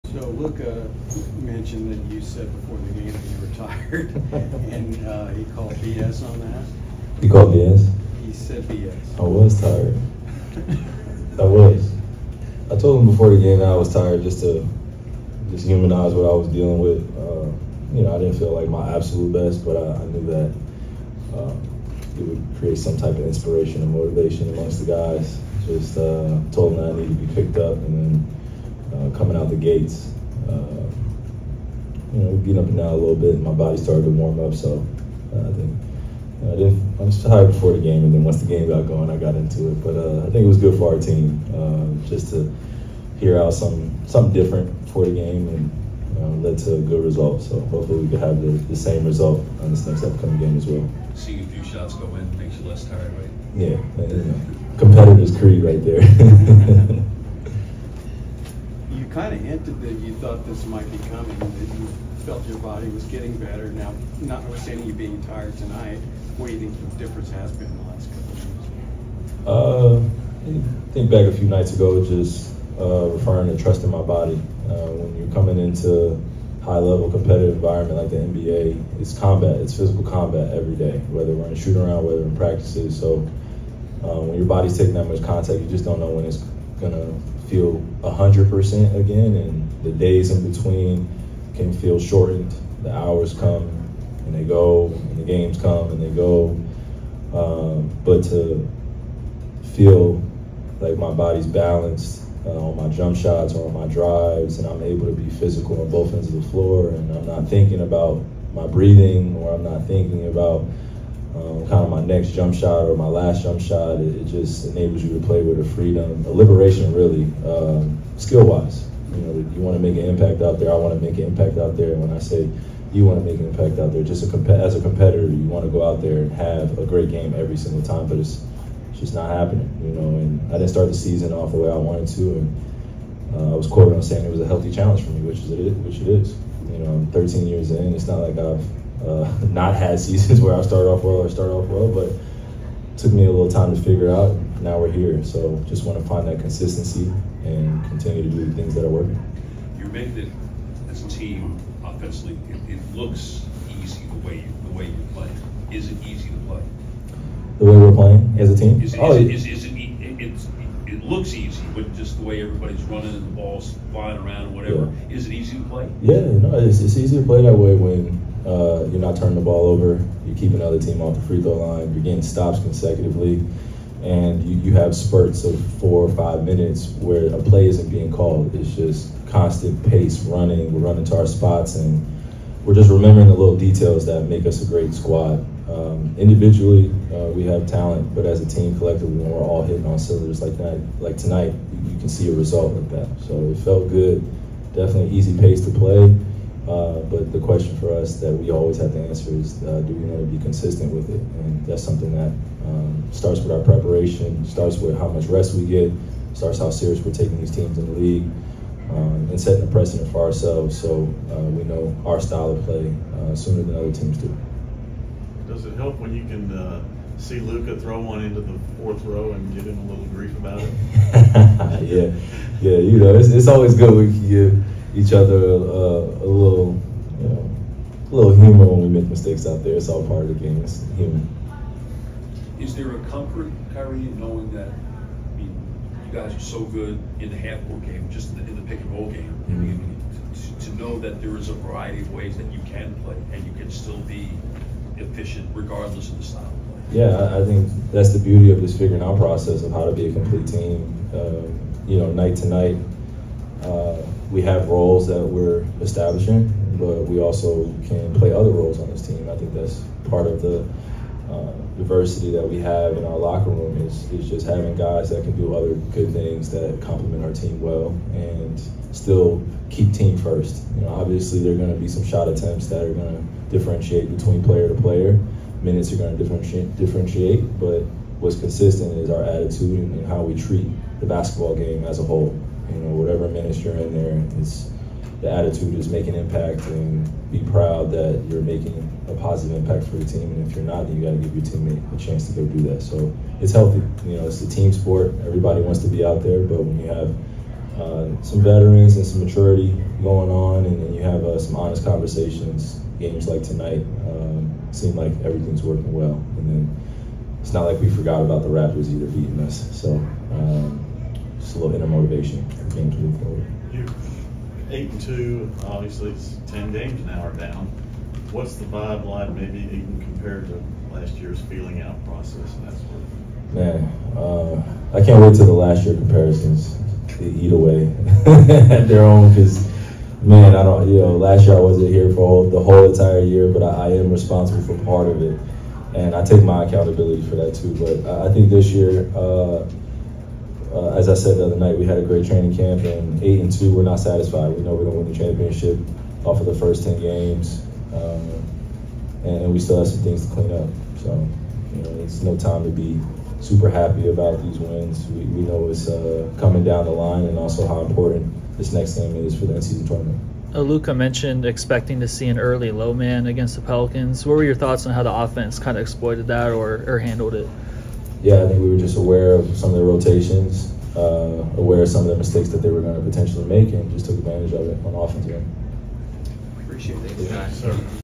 Kyrie Irving Speaks After Mavs' Win vs. Pelicans: Nov. 12, 2023
Dallas Mavericks superstar Kyrie Irving spoke to reporters after the team's 136-124 victory over the New Orleans Pelicans. He finished with 35 points, 6 rebounds, and 7 assists.

Check out my full Dallas Mavericks coverage for daily updates, analysis, and exclusive interviews.